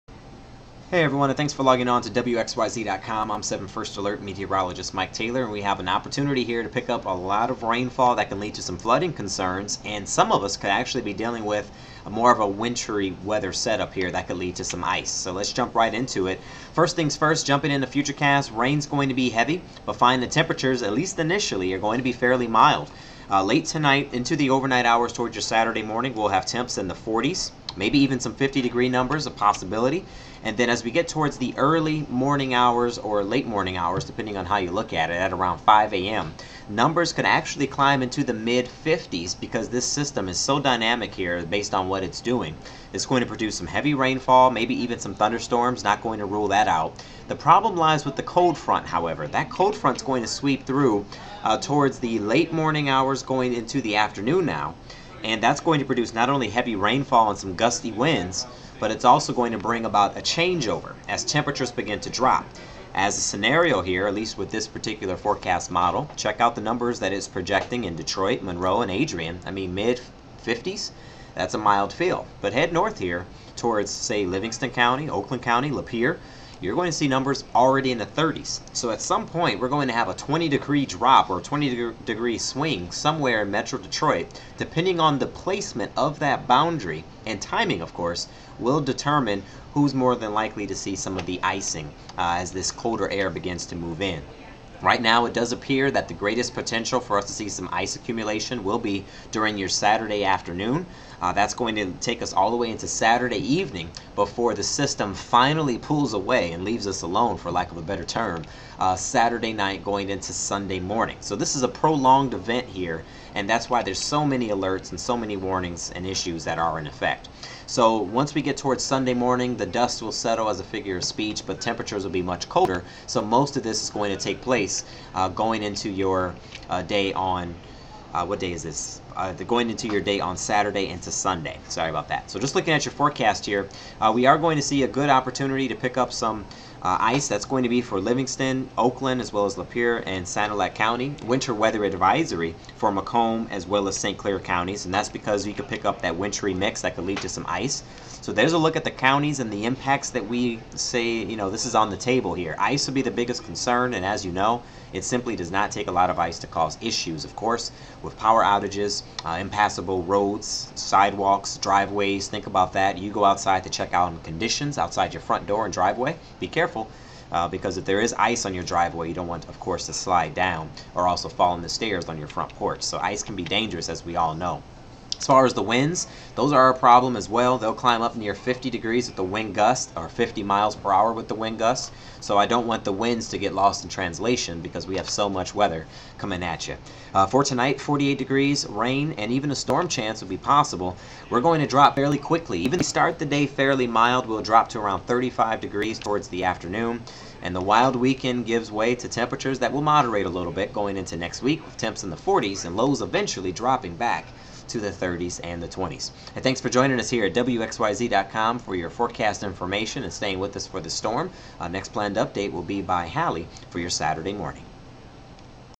Winter storm this weekend
A strong winter storm takes aim at Metro Detroit this weekend. 7 First Alert meteorologist Mike Taylor has the latest.